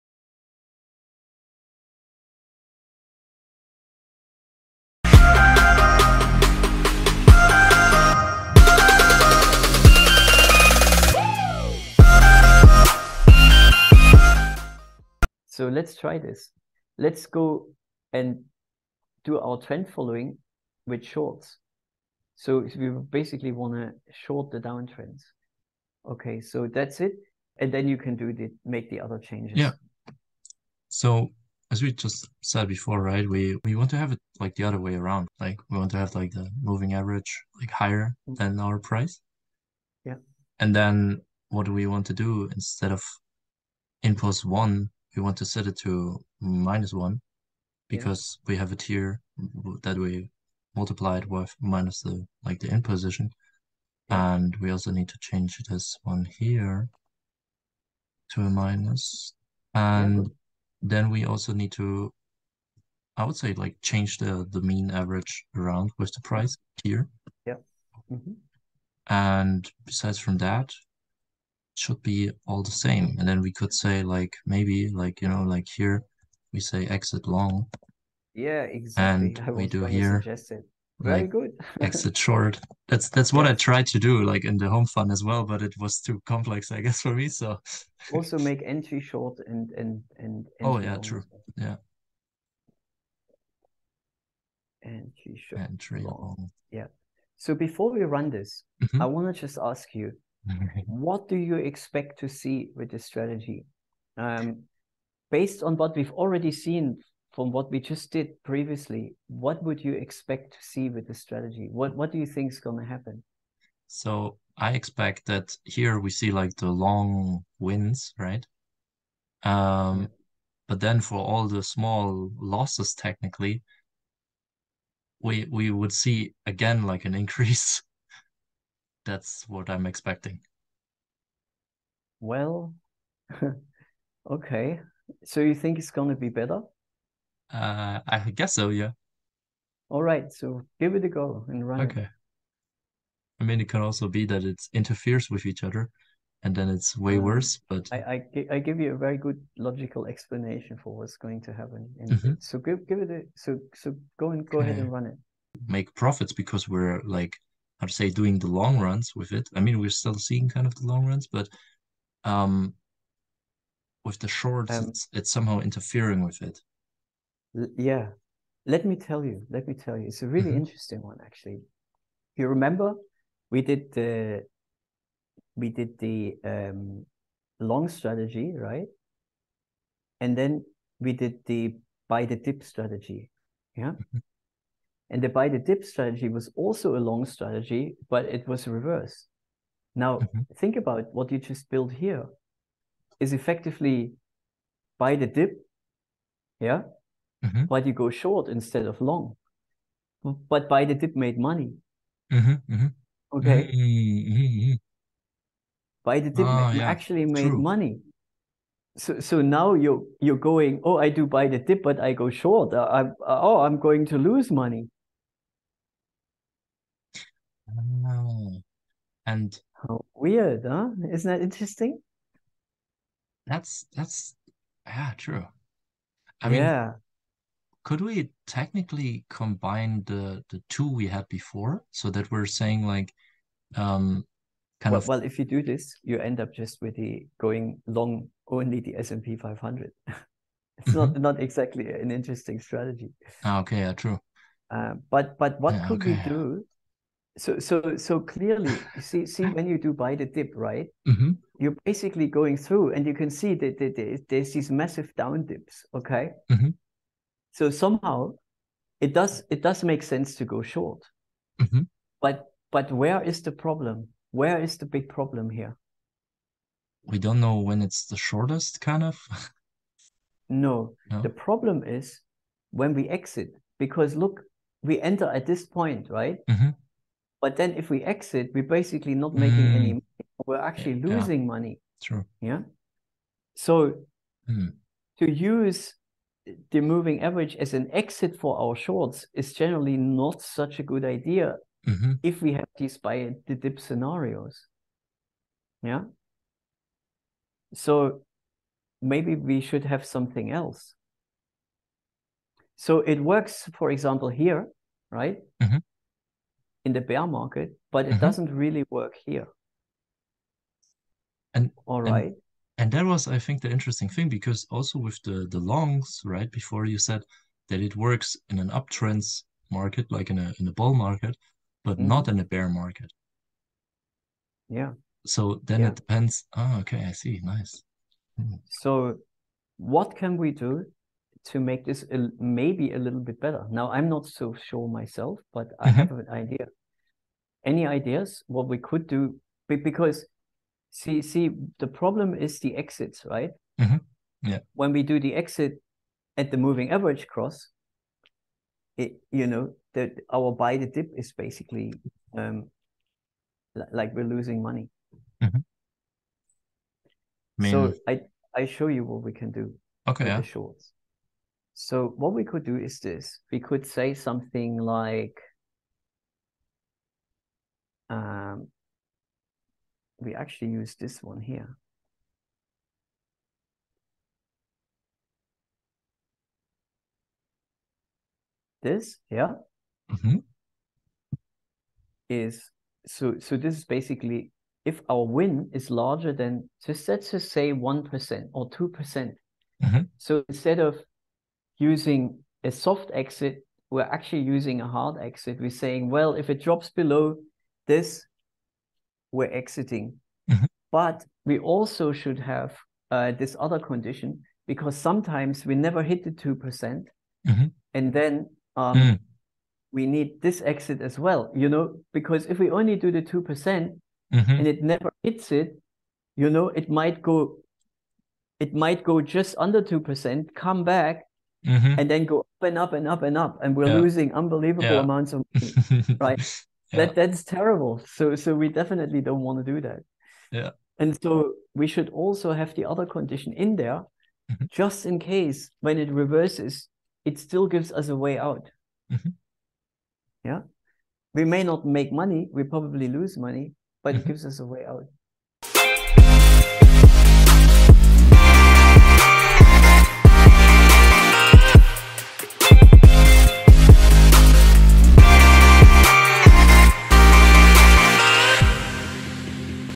Maximize Profits By Shorting The Downtrend | Algo Trading | Python
‎ You will learn why hard exits are important for shorting and when the mean reversion strategy doesn't work anymore.